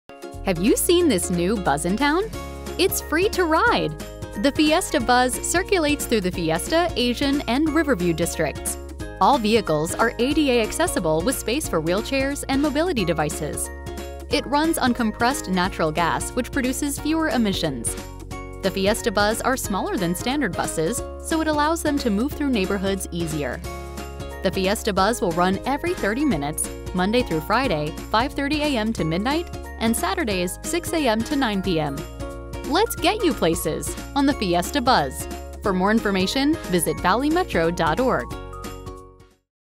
What are the features on the Fiesta Buzz?
It's a new bus circulator in west Mesa. It's free to ride!